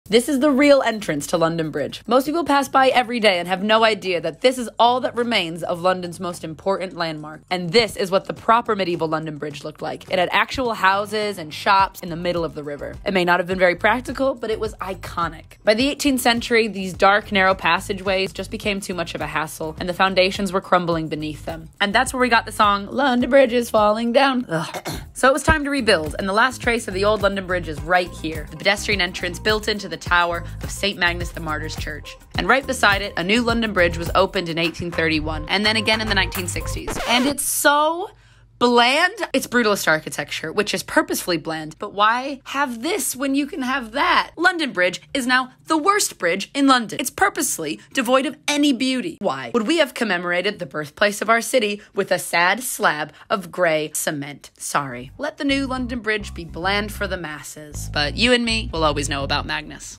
This Is The REAL London Bridge
Forgive my rage. The original medieval London Bridge was much cooler and a little piece of it still remains hidden in this churchyard,